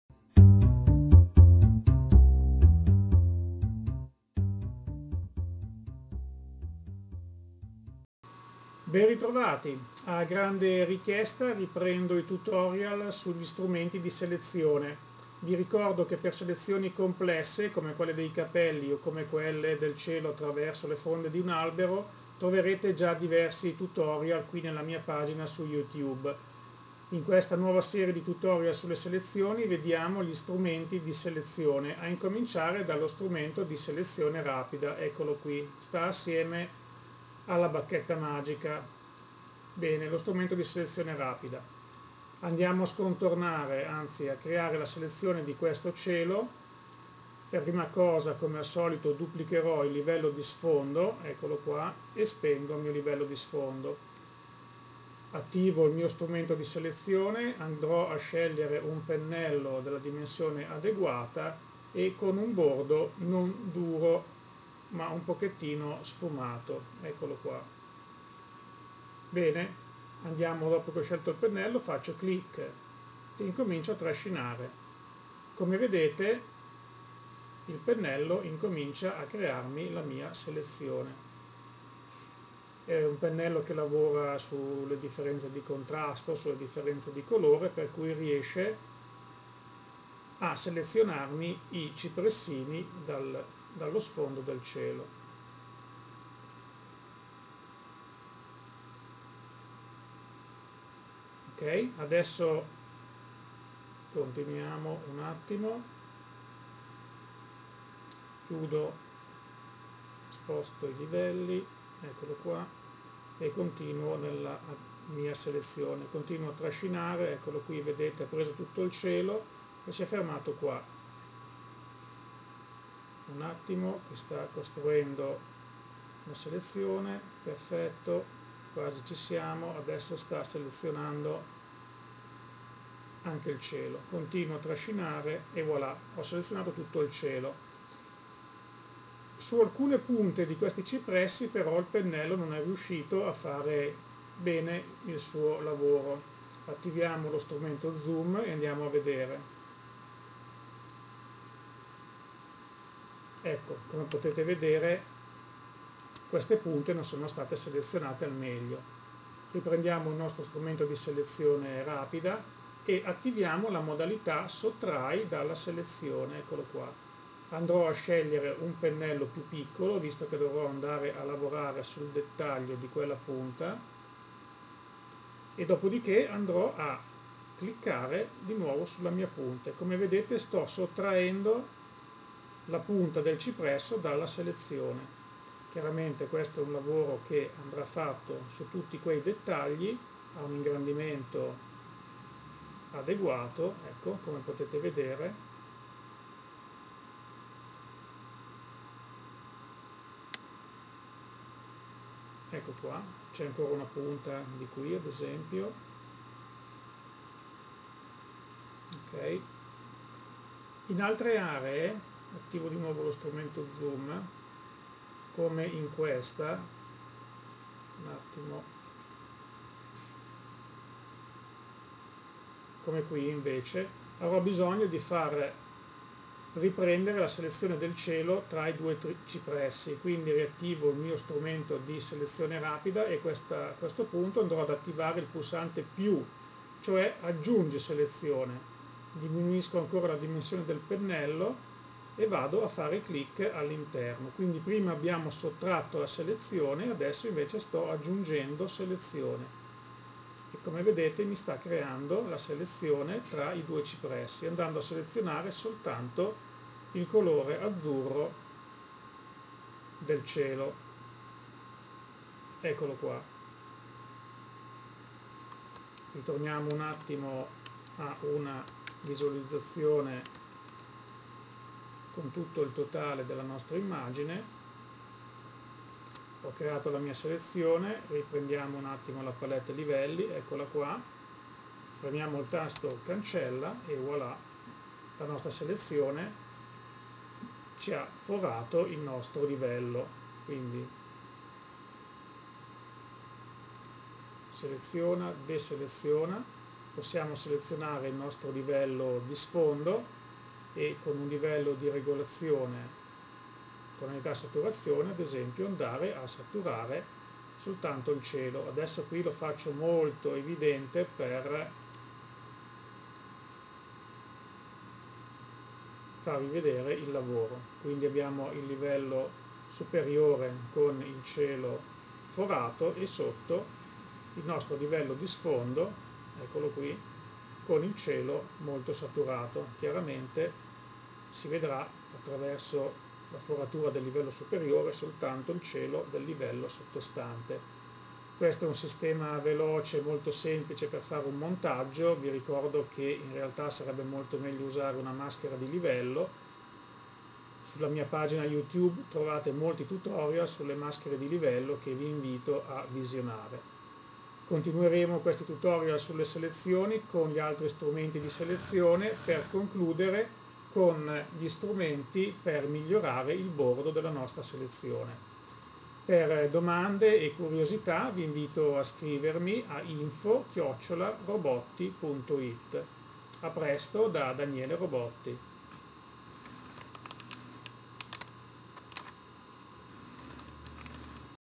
Photoshop: selezione rapida
Tutti gli strumenti per selezionare, primo tutorial dedicato a Selezione Rapida. Come funziona e come fare una buona selezione in pochi click lo strumento Selezione Rapida.